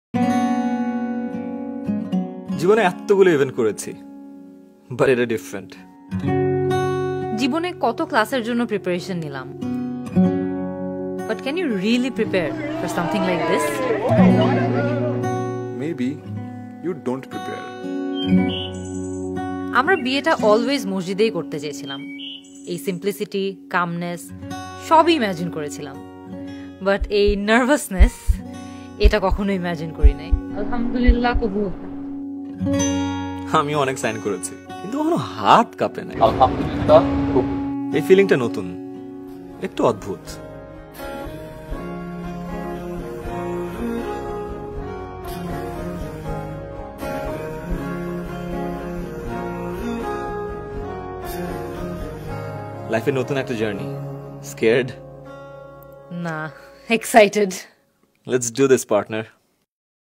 একটা অন্যরকম বিকেল। একটা অন্যরকম পথচলার শুরু। আয়মান সাদিক - মুনজেরিন শহীদ ❤️ | WEDDING ART SYLHET
Ayman Sadiq weds Munzereen Shahid ❤️
একটা অন্যরকম বিকেল। একটা অন্যরকম পথচলার শুরু। ❤️
Video collecting from Ayman Sadiq's page